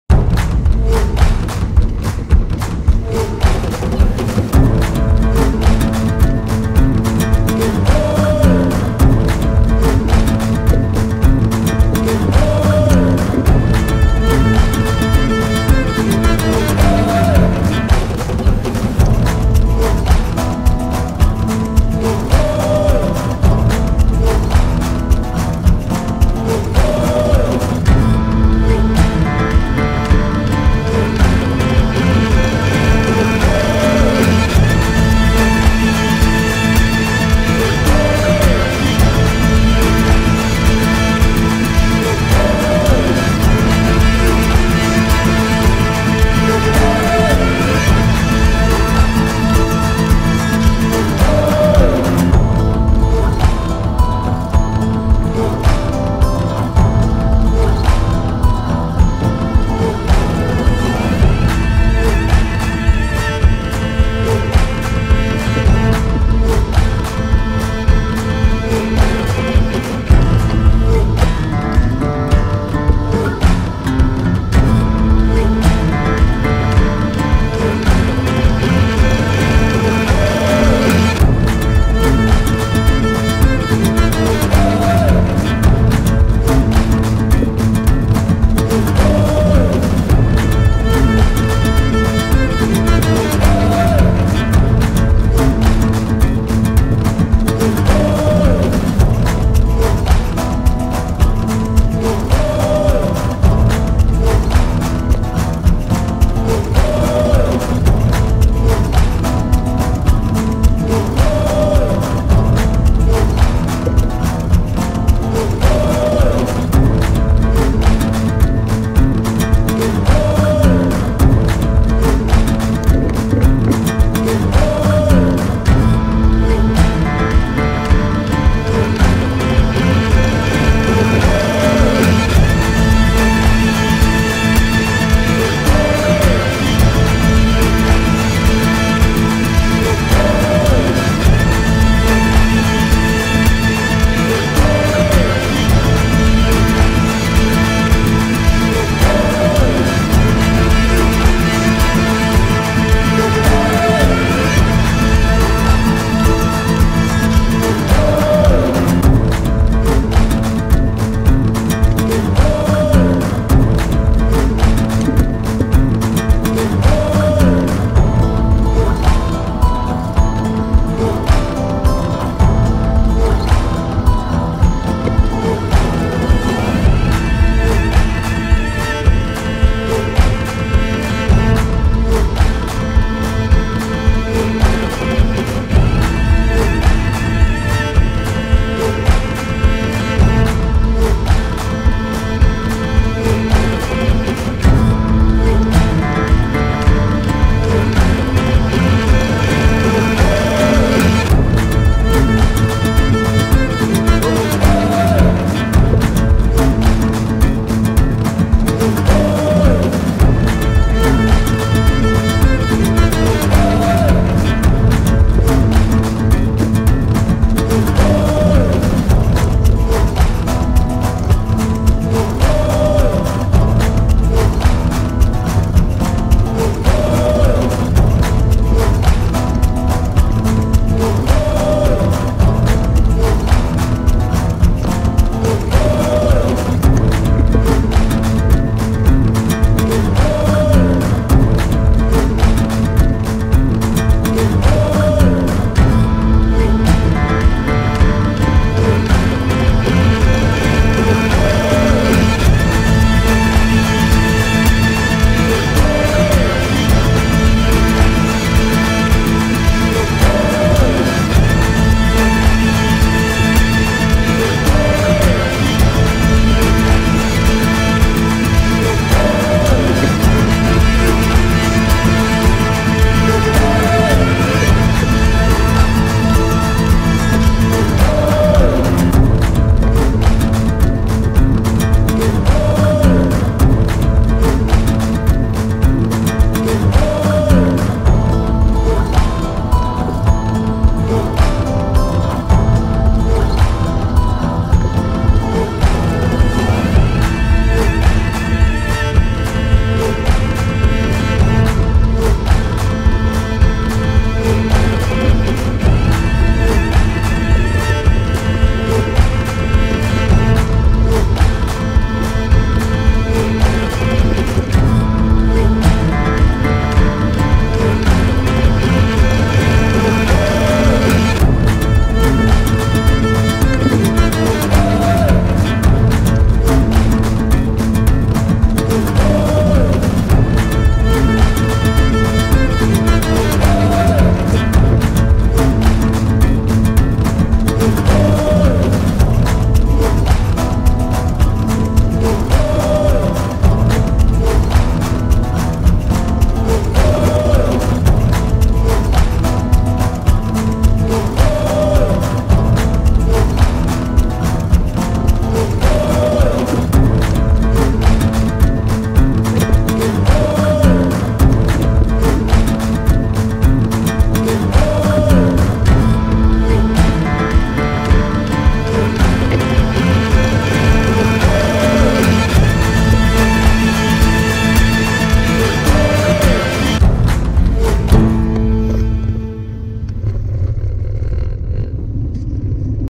Up and down the creak at Rhodes
Rhodes single track with some mountain bike buddies.